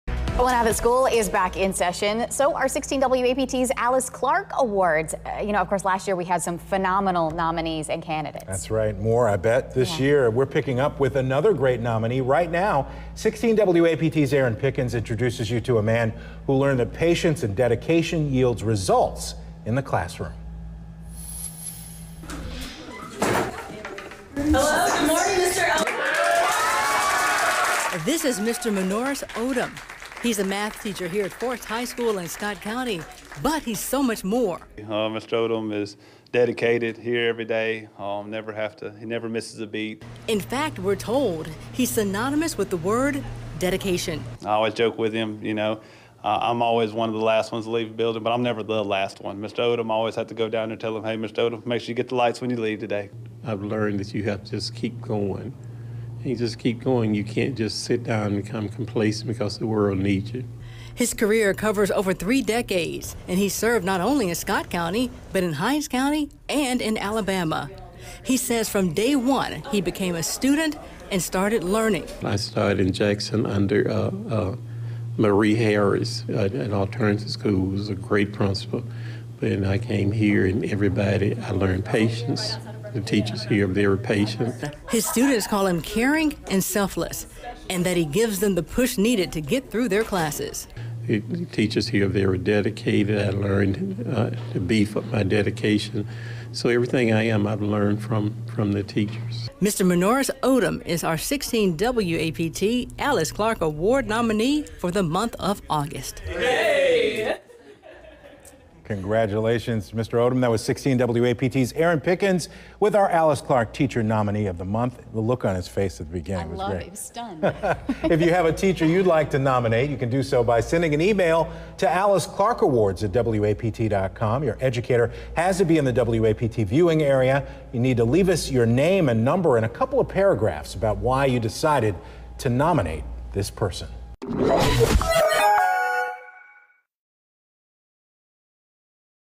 Mannorris Odom wins latest Alyce Clarke Award
After 30 years in the classroom, math teacher’s dedication hasn’t waned